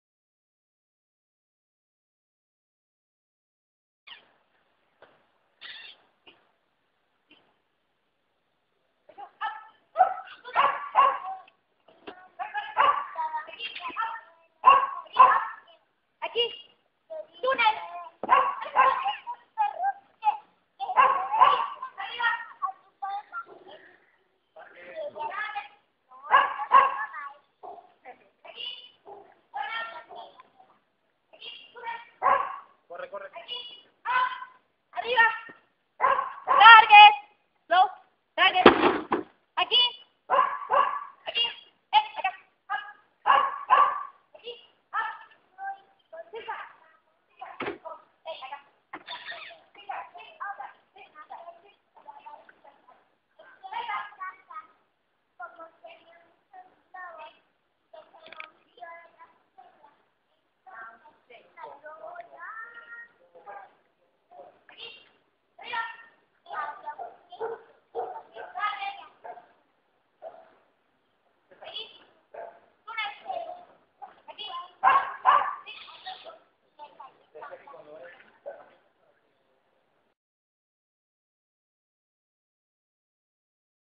Entrenamiento Agility junio 13 2009
Entrenamiento de Mushu, con la voz de Luisita como fondo...